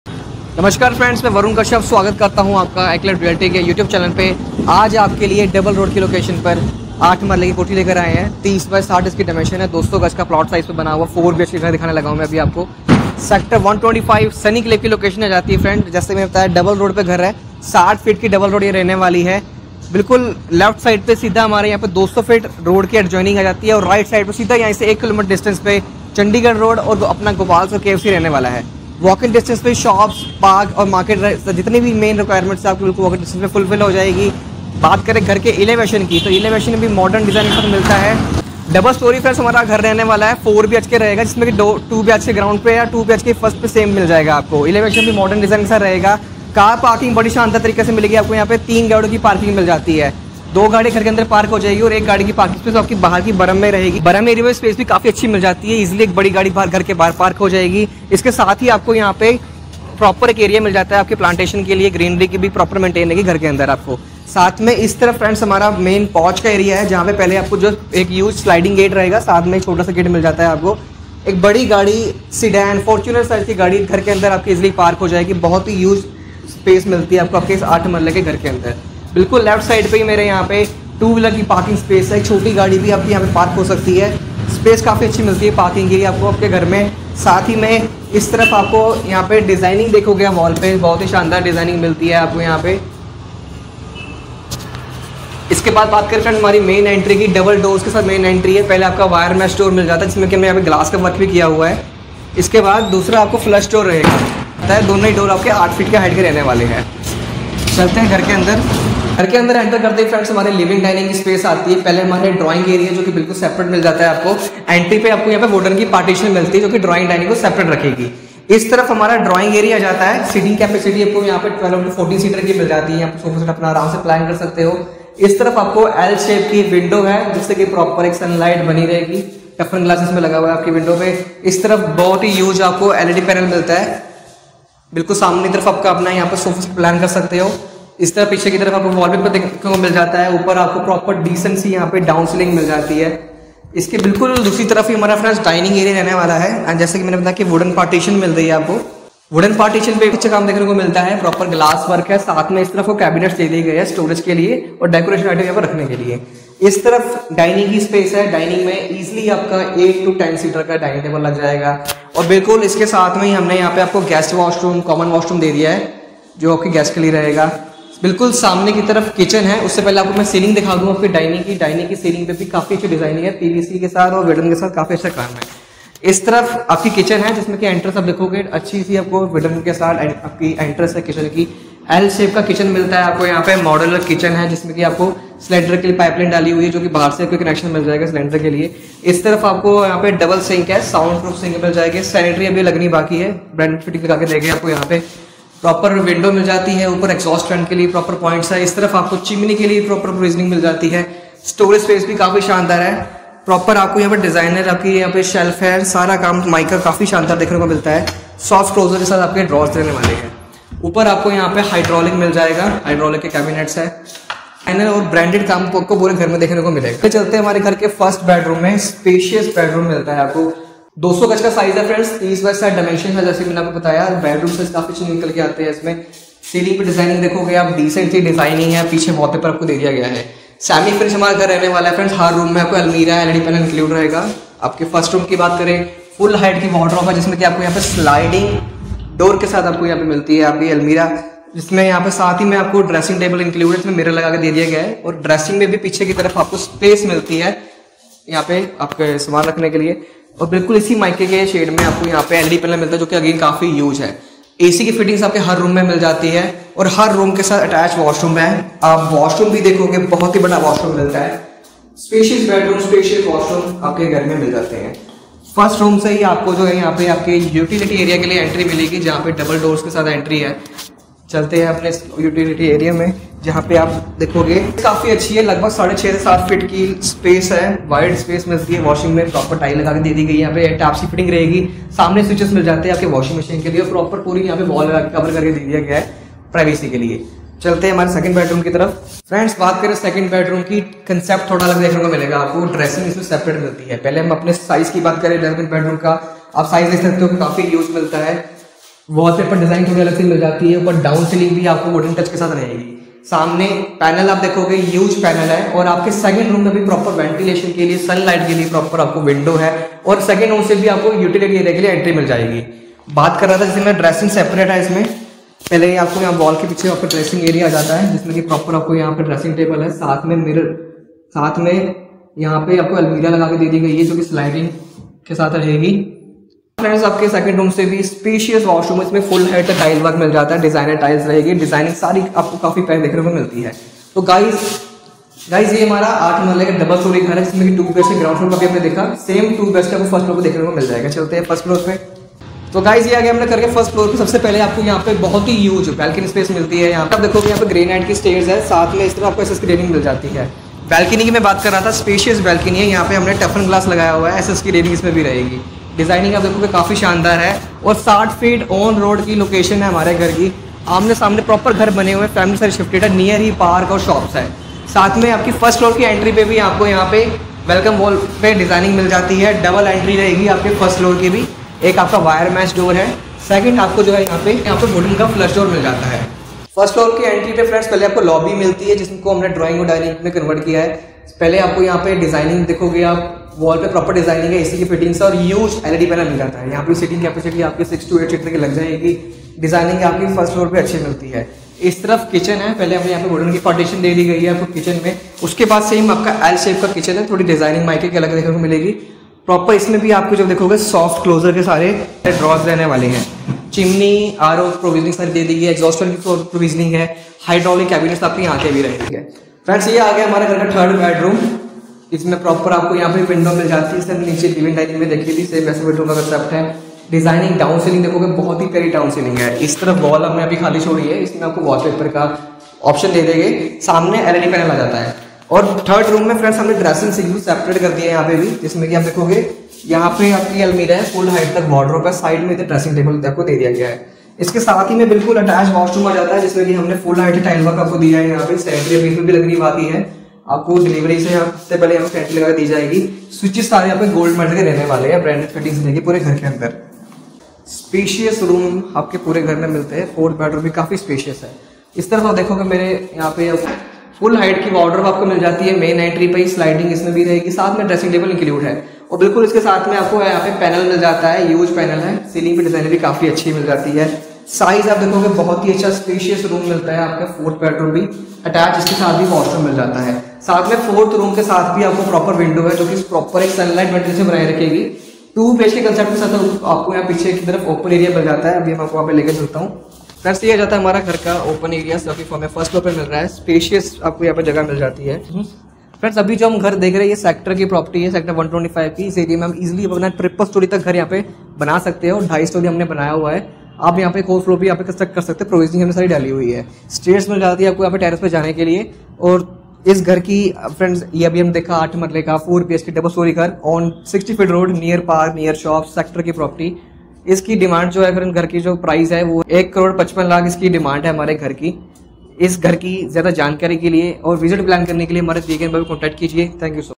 मोहाली में 200 गज का घर | 4BHK East Facing Kothi | On 60ft Road
200 Sqyards 4BhK Independent house for sale
3 car Parking • East Facing
On 60 ft Double road Adjoining 200 ft road
near Nijjar chowk and Airport Road
Sector 125 Sunny Enclave Mohali